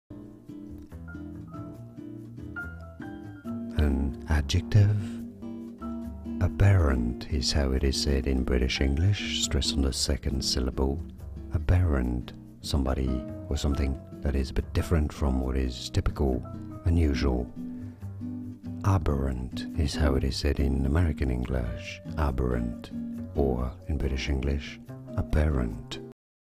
How to Pronounce Aberrant
This video shows you How to Pronounce Aberrant, pronunciation guide.
00:00 - Pronunciation Intro
00:08 - How to Pronounce Aberrant
00:20 - More Difficult Pronunciations